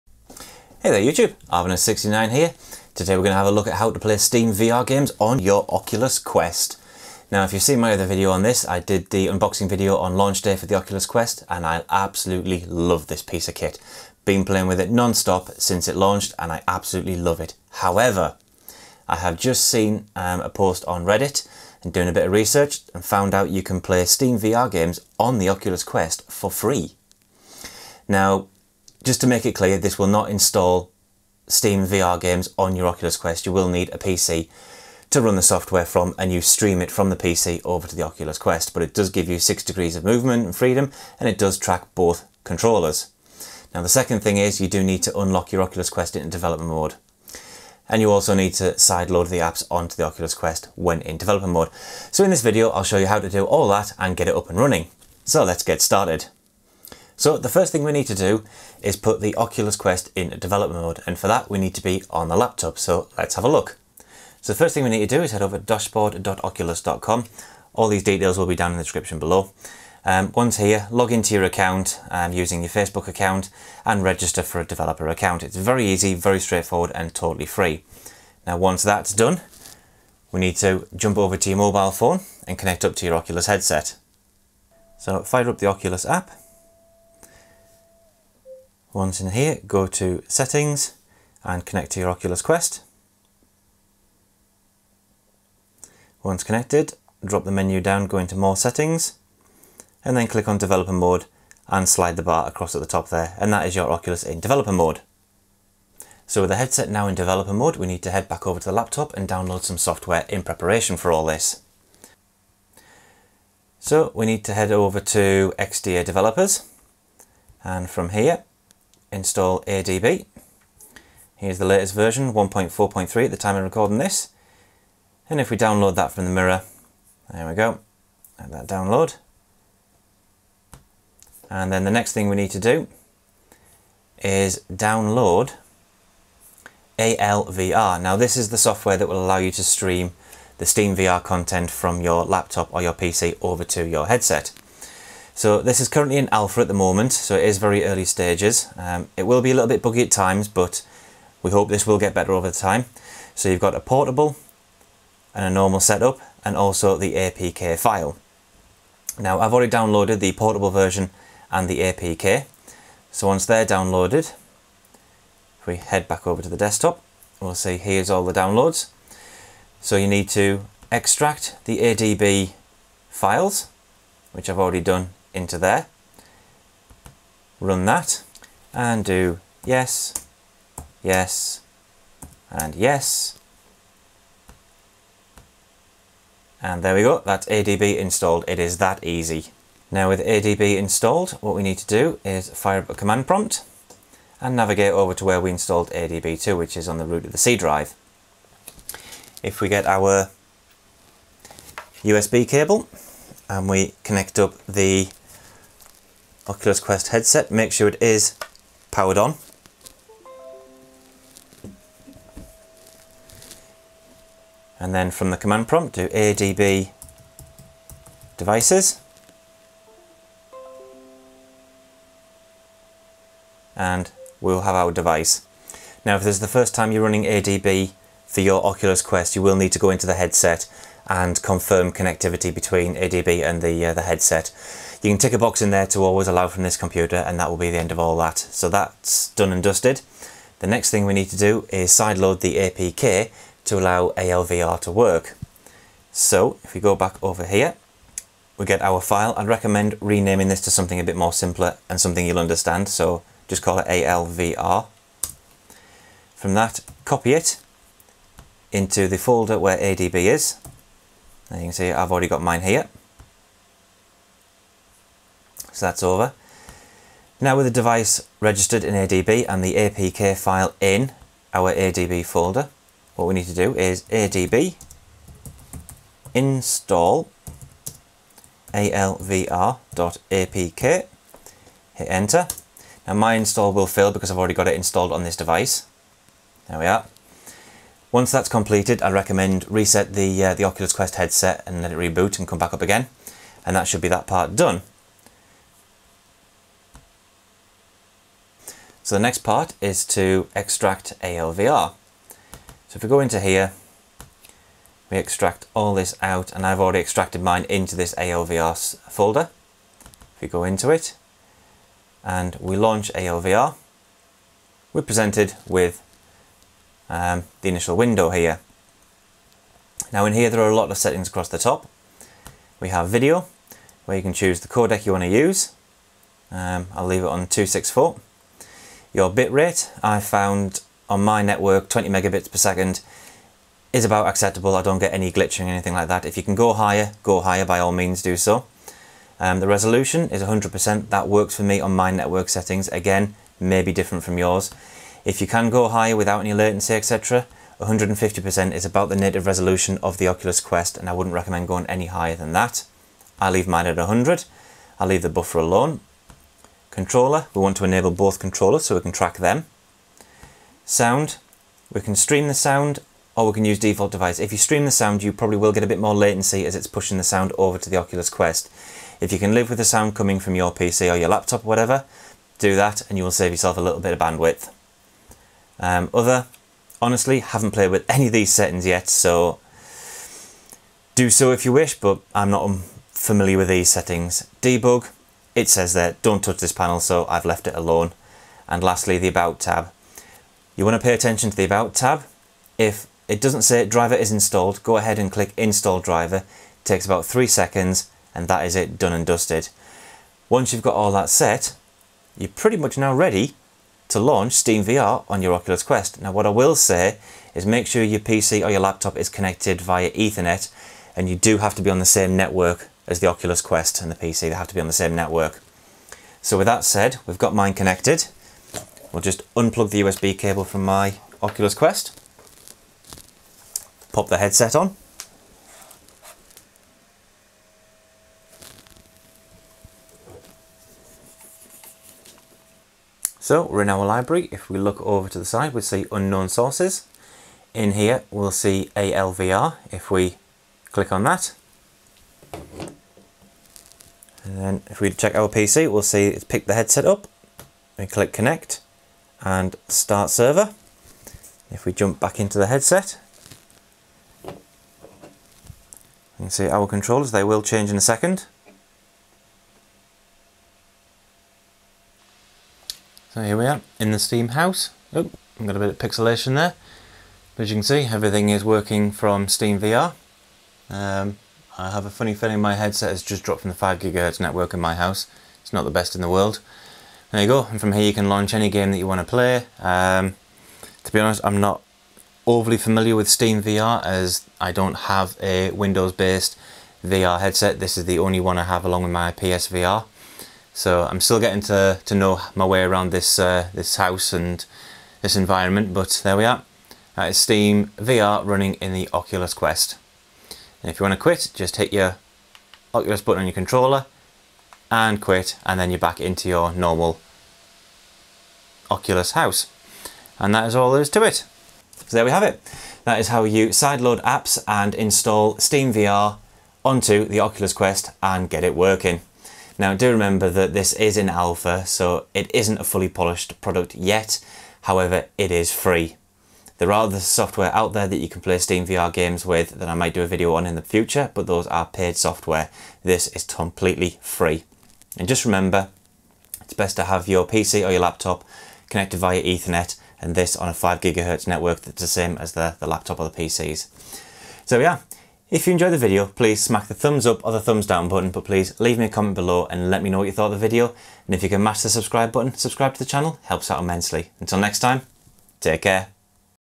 How to Play SteamVR on the Oculus Quest
After reading on Reddit and twitter about people playing SteamVR on the Oculus Quest I was intrigued and wanted to give it a go. The ability to play high end PC VR games on a completely wiresless headset with full 6 degrees of freedom was too good to miss. Also not to mention that at a starting price of £399 it's got to be worth a try.
In this video I show you how to put the Oculus Quest in to Developer mode. How to side load the ALVR app and set it all up.

You do this at your own risk and I take no responsibility for any damage that may occur

▼▼▼ Website's ▼▼▼

▼▼▼  Social Media ▼▼▼